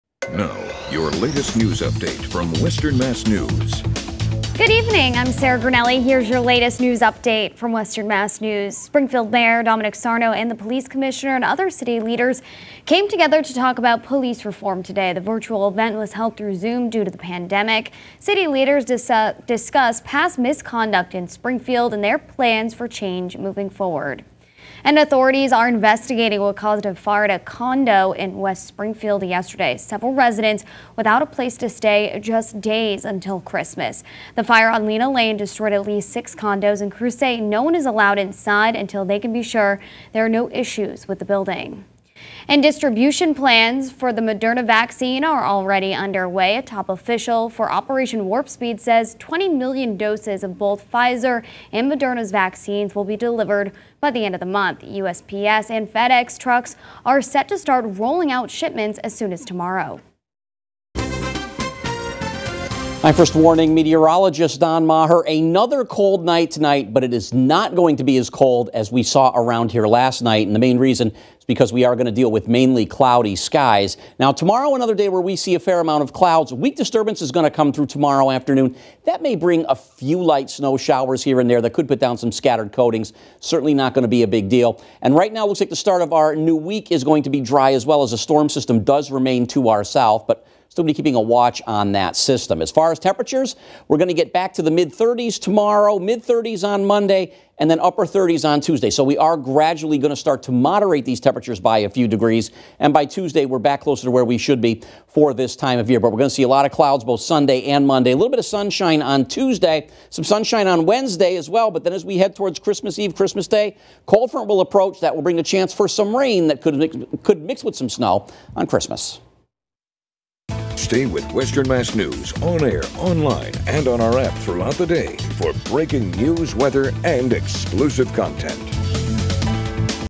Saturday Night News Update
In this news update, the Springfield mayor, police commissioner, and other city officials came together to talk about police reform during a virtual conference where they discussed past misconduct that took place across the city. Also, authorities investigating what caused a major fire at a condo complex in West Springfield late Friday night. Meteorologist Don Maher says to expect dry weather tonight with a chance of showers by tomorrow afternoon.